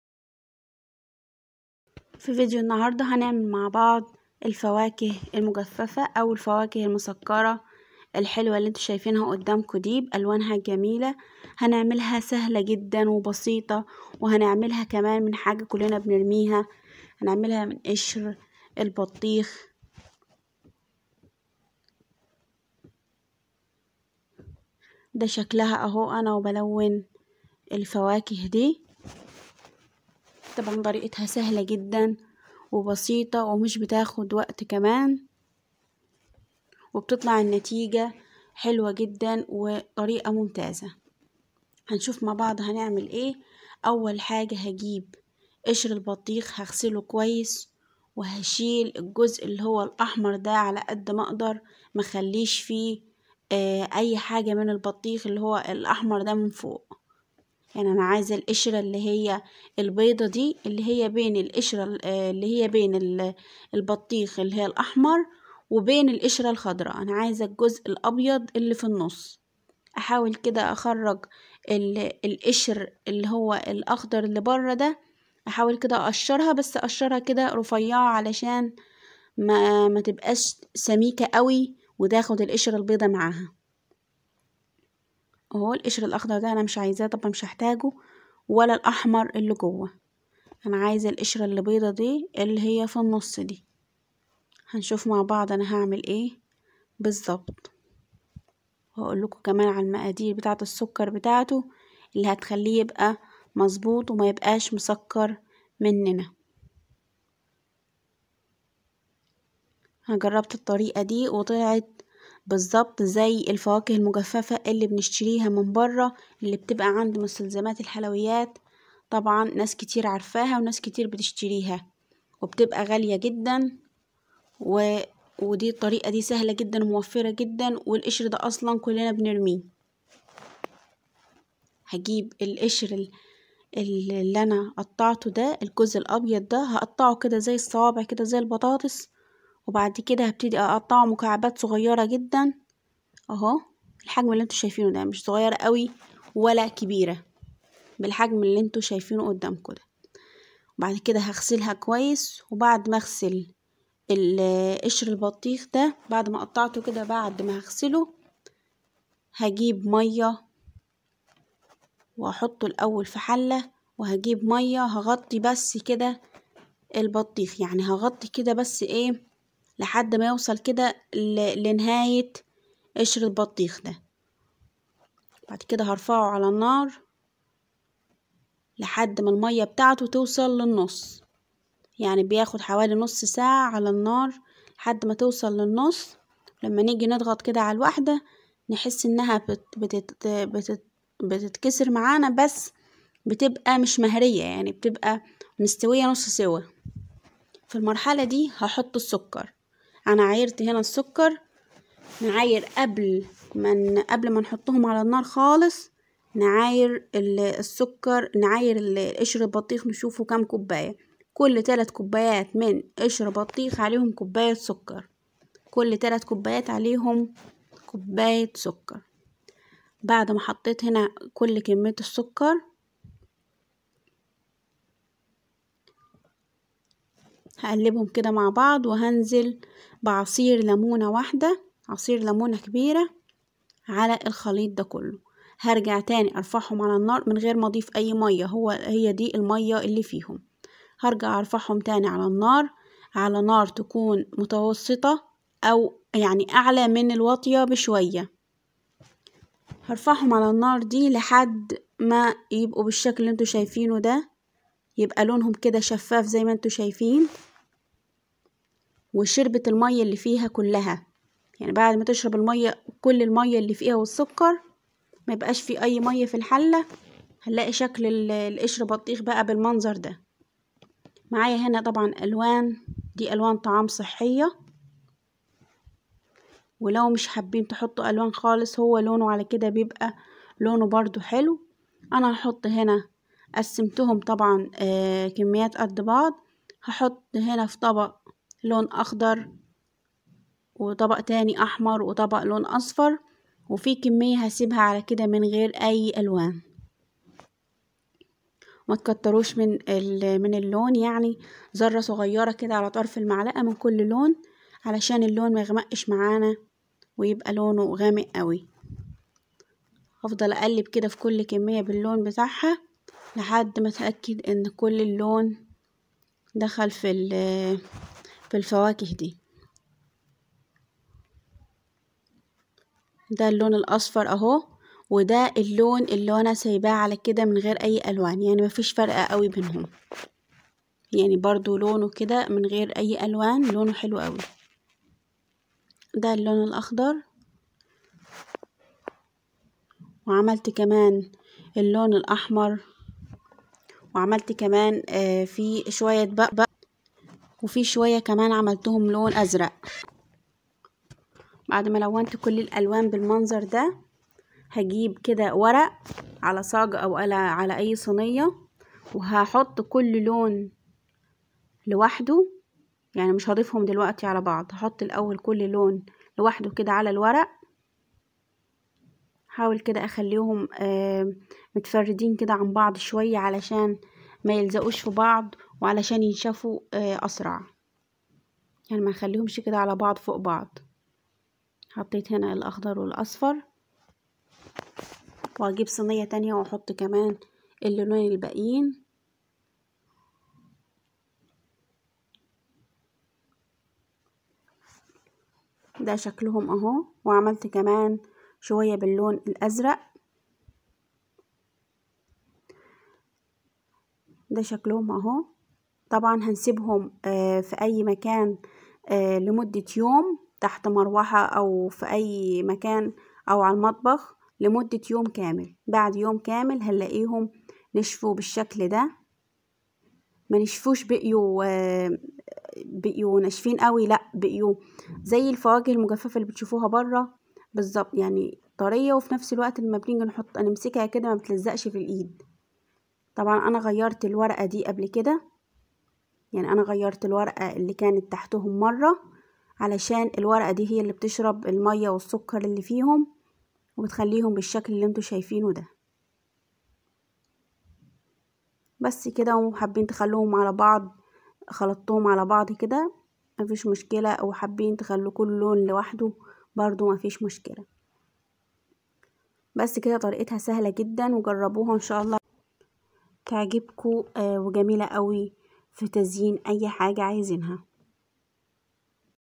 باسهل الطرق عملنا فواكه مجففة لتزيين التورت والايس كريم🍡🍒 / فواكه مسكرة من قشر البطيخ🍉
من قشر البطيخ اللى بنرمية هنعمل بيه حجات حلوة جدا لتزيين كل انواع الجتوهات والايس الكريم بطريقة سهلة جدا وبسيطة .
قبل رفع قشر البطيخ على النار نعايرهم بالكوباية
كل 3 كوبايات من قشر البطيخ عليهم كوب سكر واحد
لو حابين تقللوا السكر حسب الرغبة.
ابتكارات من قشر البطيخ